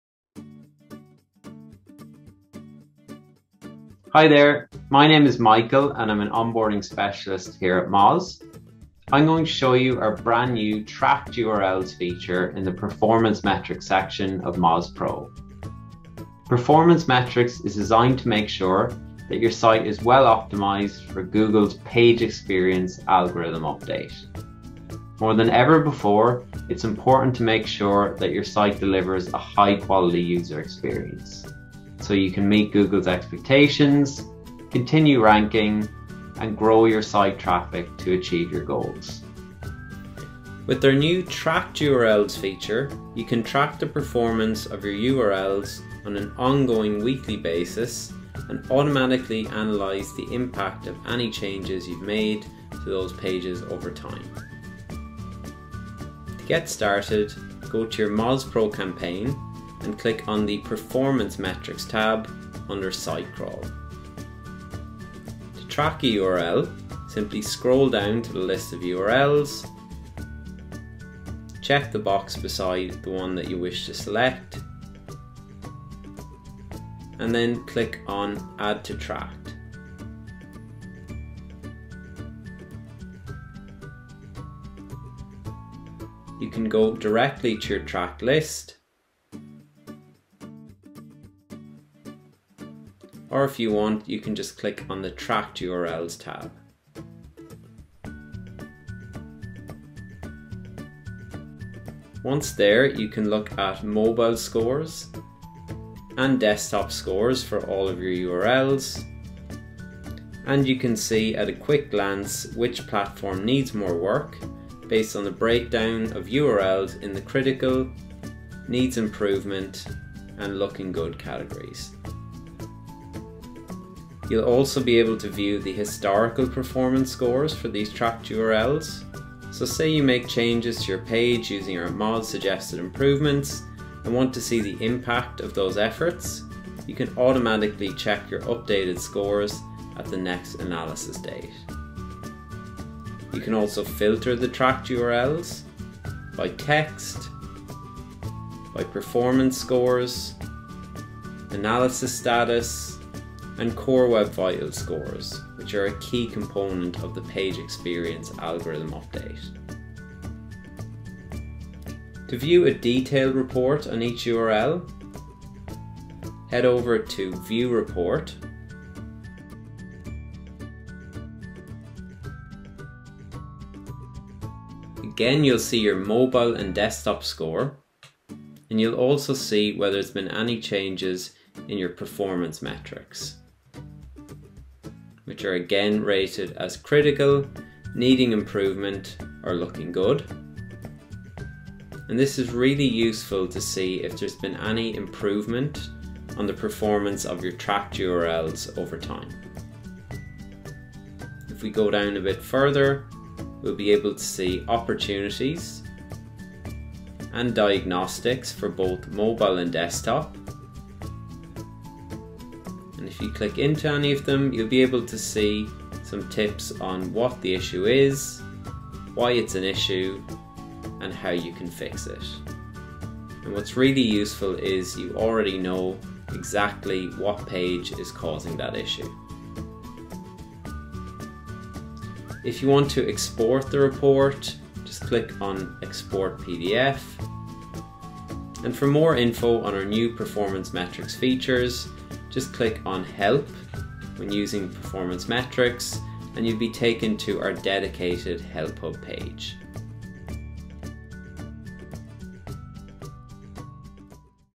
Automatically Analyze Tracked URLs with Performance Metrics
Our new Performance Metrics tool allows you to automatically analyze tracked URLs to ensure you're on track for your Core Web Vitals. Join Michael from our Onboarding Team as he talks you through the many benefits automated analysis could have on your sites.